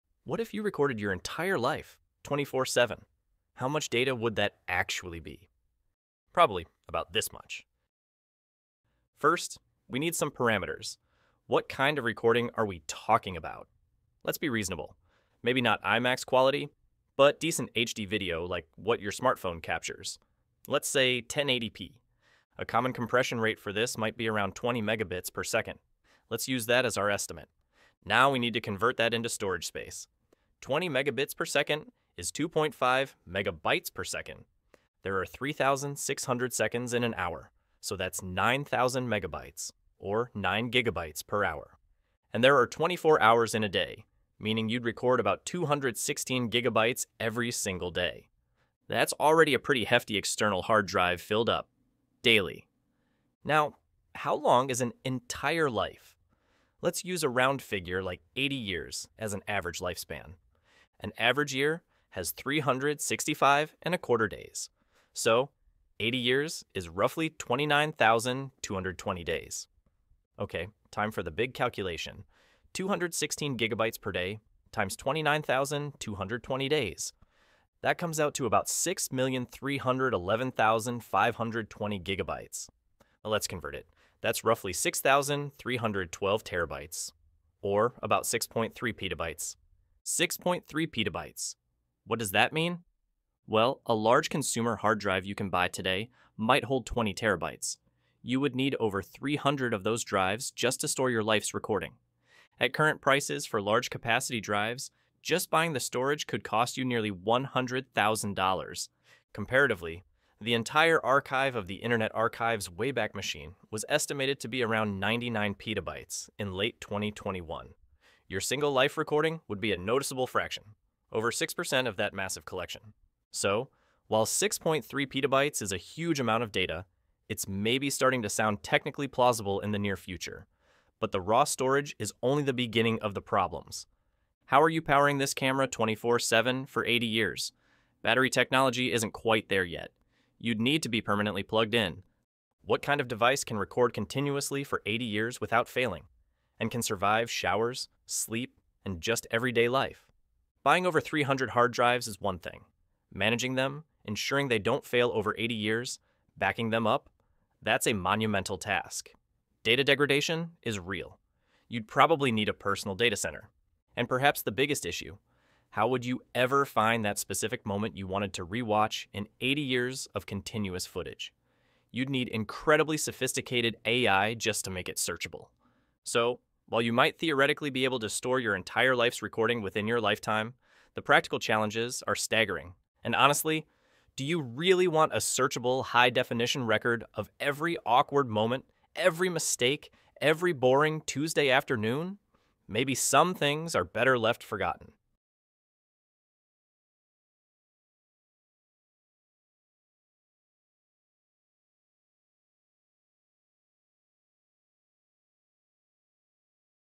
What if you recorded your entire life?
Could you record your entire life, 24/7? How much data would it take?